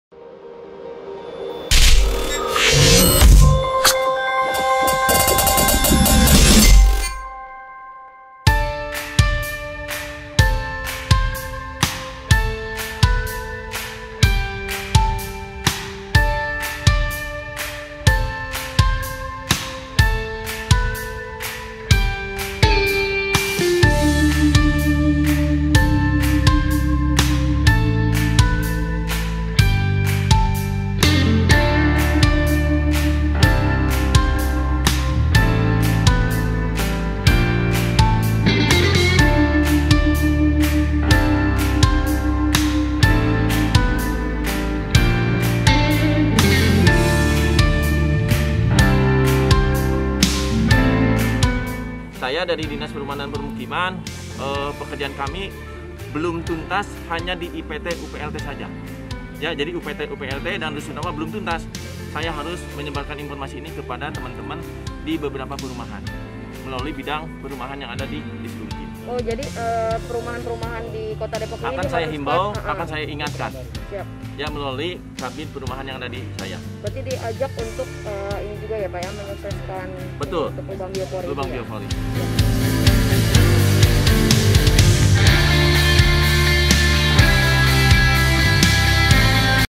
Disrumkim Komitmen Lestarikan Lingkungan Lewat Pembuatan Lubang Biopori
berita.depok.go.id - Dinas Perumahan dan Permukiman (Disrumkim) Kota Depok berkomitmen untuk ikut melestarikan lingkungan melalui pembuatan lubang biopori. Saat ini pihaknya telah membuat sebanyak 21 lubang biopori di Unit Pelaksana Teknis (UPT) Instalasi Pengolahan Limbah Terpadu (IPLT).

"Sekarang sudah ada 21 lubang biopori di IPLT Depok. Kami akan sebarkan program ini ke seluruh masyarakat melalui pegawai IPLT," tutur Kepala Disrumkim Kota Depok, Dudi Mi'raz kepada berita.depok.go.id, Selasa (16/03/21).

Dudi mengatakan, pembuatan lubang bipori memiliki banyak manfaat. Salah satunya untuk menyerap air saat hujan sehingga mencegah terjadinya genangan atau banjir.

“Lubang biopori juga baik untuk menyimpan sampah organik sisa makanan yang bisa dijadikan kompos,” ujarnya.

Dia menuturkan, penyediaan fasilitas ini guna mendukung program 100.000 lubang biopori yang dicanangkan oleh Wali Kota dan Wakil Wali Kota Depok. Kendati demikian, imbuhnya, dibutuhkan juga partisipasi masyarakat agar program tersebut dapat terwujud.

"Saya berharap tokoh masyarakat menyampaikan ke warganya supaya membuat lubang biopori minimal satu rumah dua lubang, ini gerakan kecil namun besar manfaatnya," katanya

Di tempat yang sama, Kepala UPT IPLT Kota Depok, Burhanudin menambahkan, dari 21 lubang biopori yang dibuat, 12 di antaranya digunakan sebagai serapan air dan sembilan lainnya untuk composting.

"Sisa makanan atau sampah organik kami akan simpan di lubang biopori ini, nantinya tanah akan menjadi subur. Gerakan ini juga akan kami lestarikan sampai ke warga setempat," pungkasnya. (VD 02/EVD 01/EUD02)